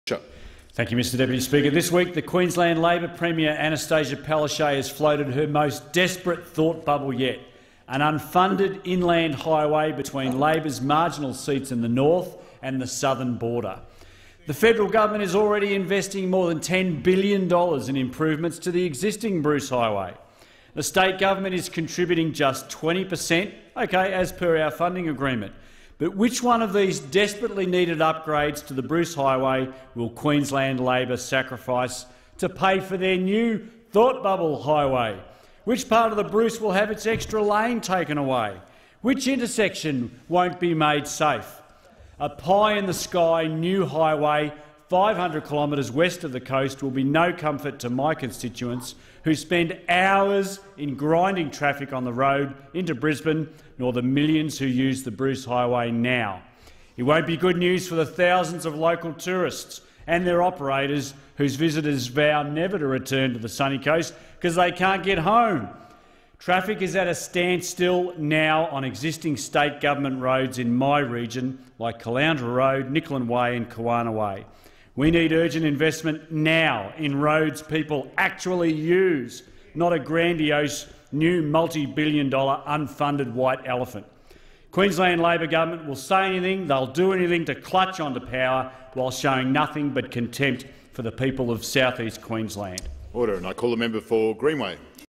Andrew Wallace MP on a second Bruce Highway
Speech in the Australian House of Representatives on 20 October 2020.